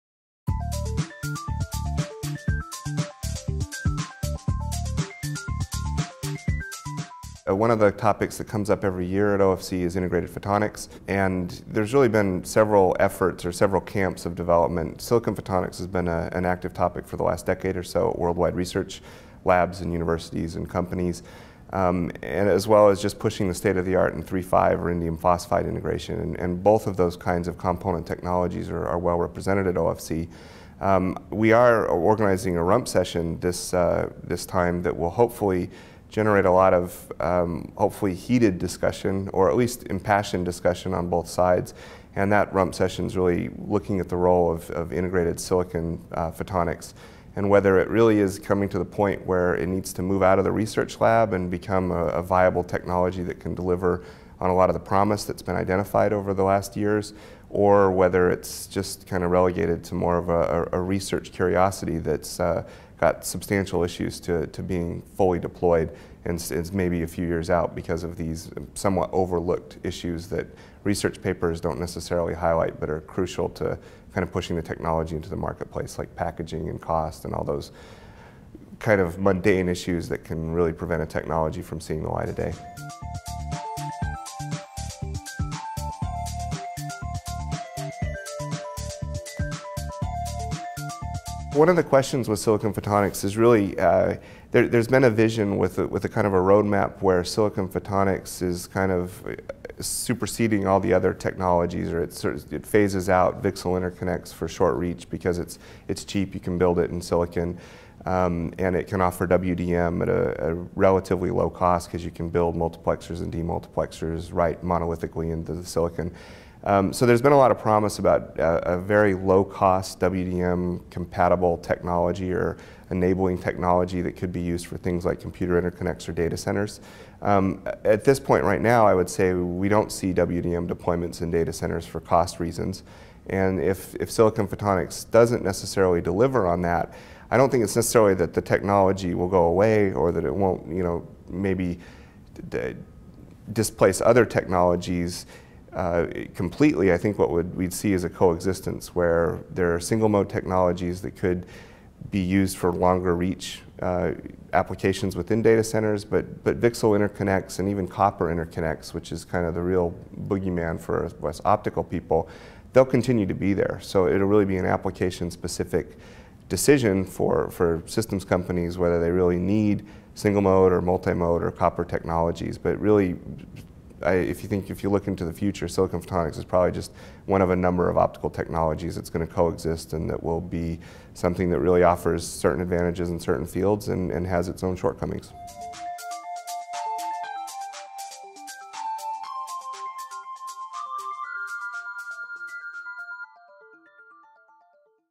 Hot Topic- Silicon Photonics: Disruptive Technology or Research Curiousity? Rump Session
Watch Hot Topic Video--Silicon Photonics by Clint Schow, IBM, USA, Program Chair